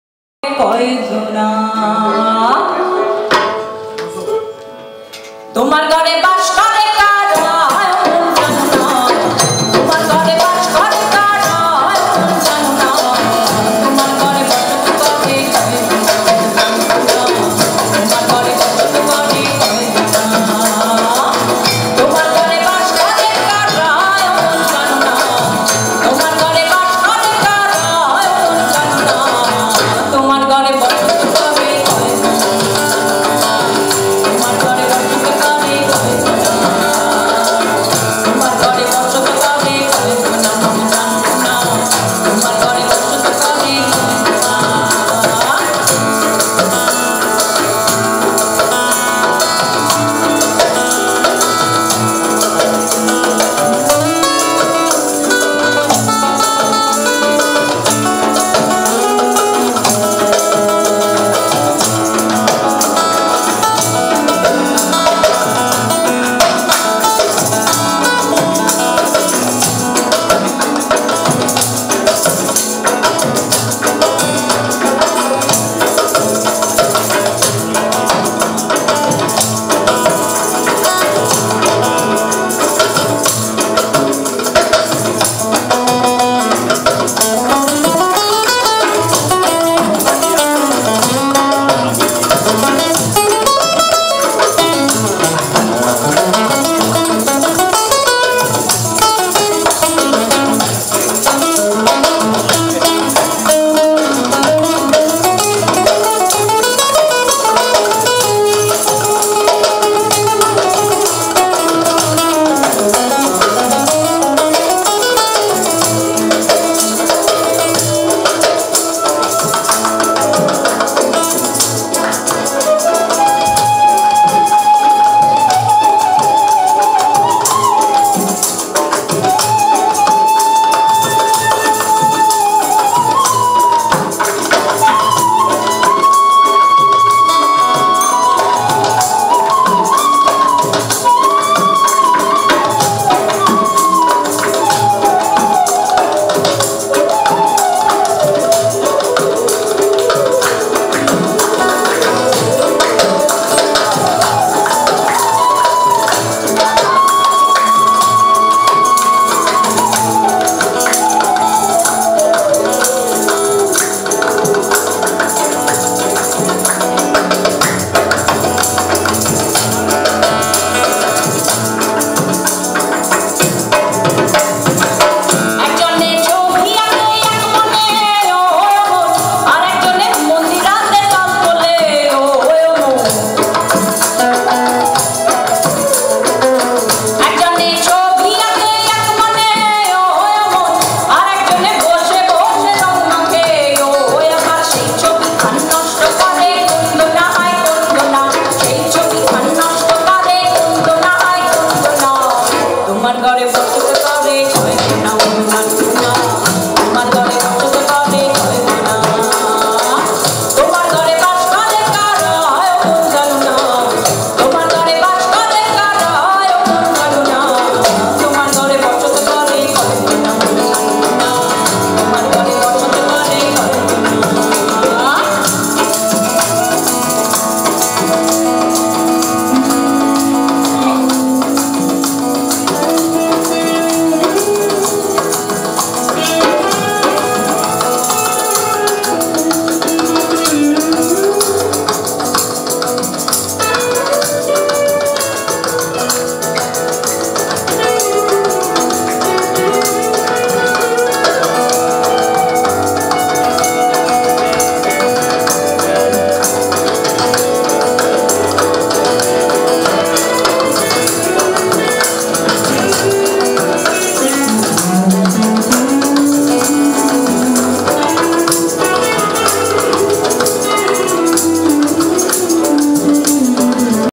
A scintillating musical evening in Panaji Goa
Fusion of Bengali Folk song & Goan guitar and percussion